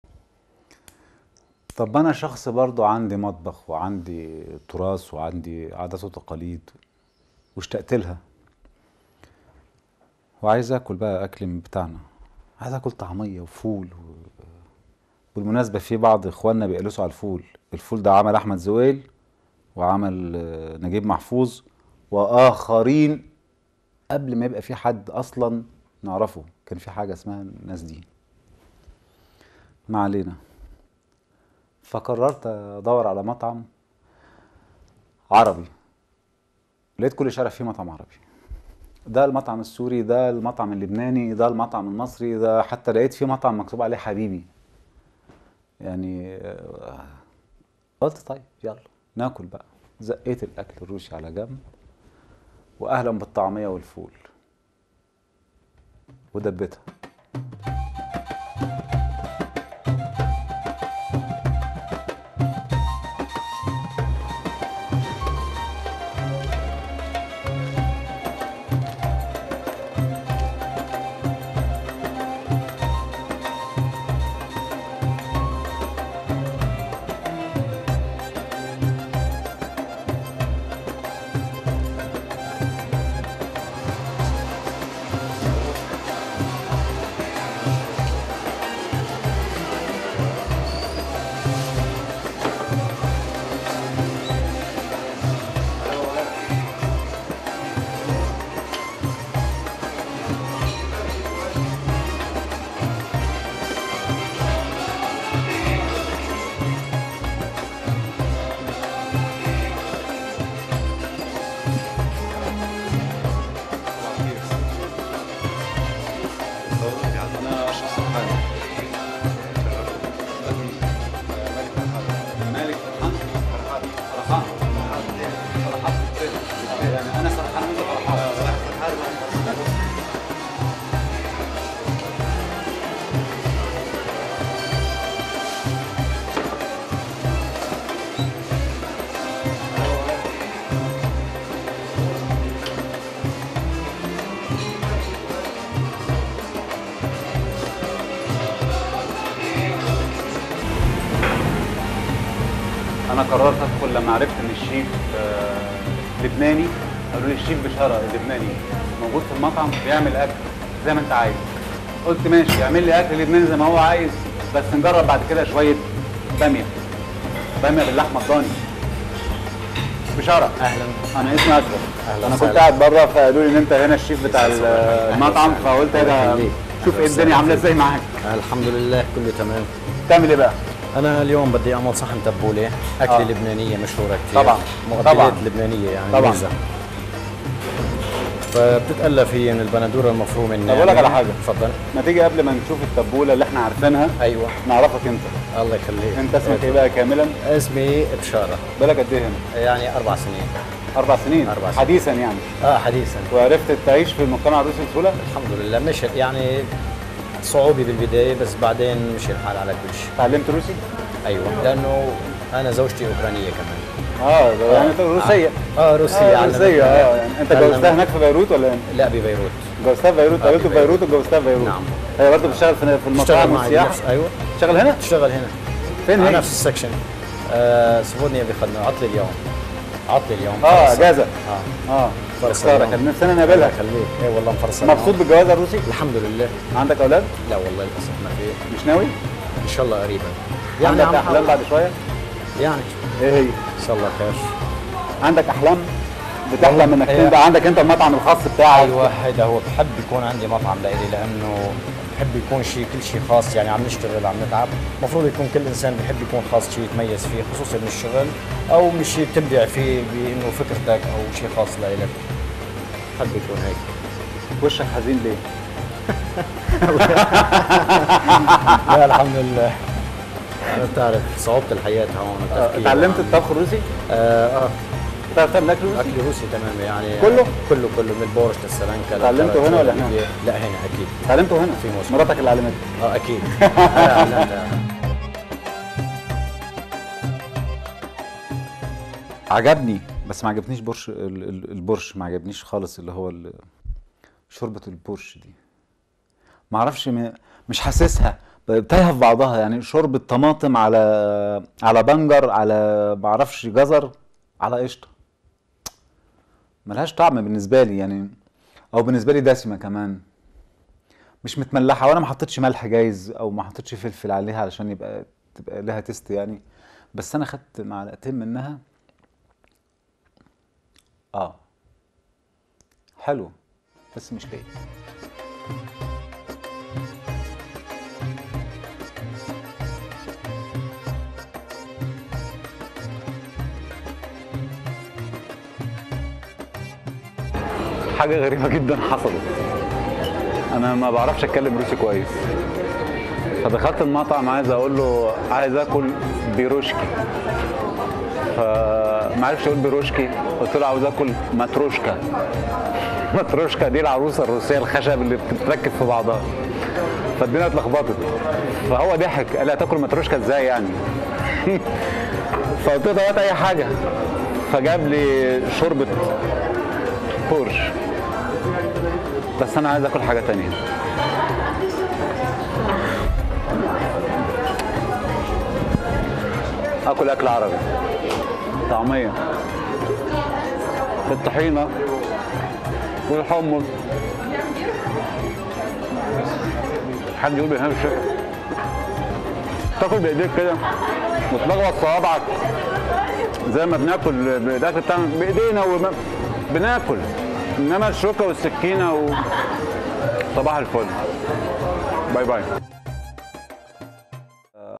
Arabian_Cuisine in Moscow
Ashraf Sarhan founds the arabian restaurants in Moscow